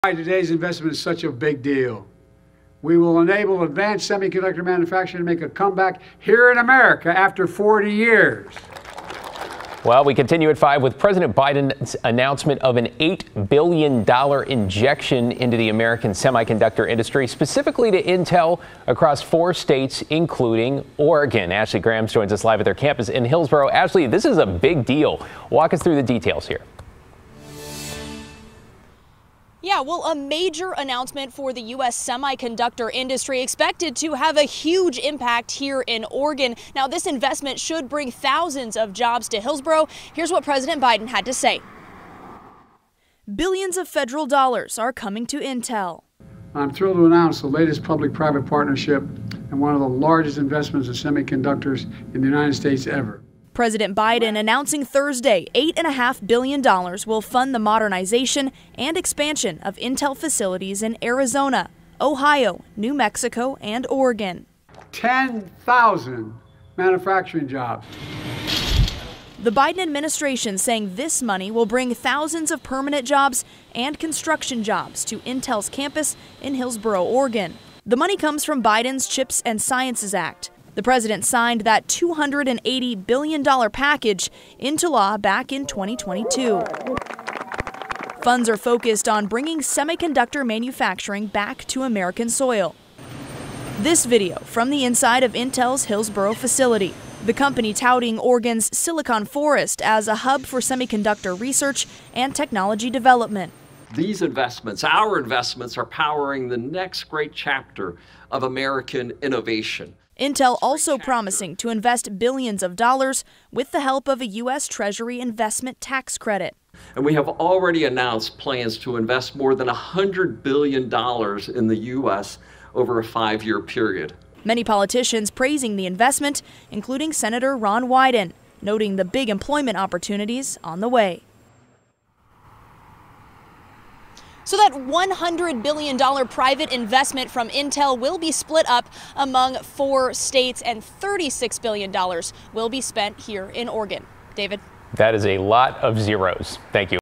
President Biden awards Intel $8.5B to expand chip making in U.S.
Intel's investment with the CHIPS Act funding is one of the largest investments announced U.S. semiconductor manufacturing.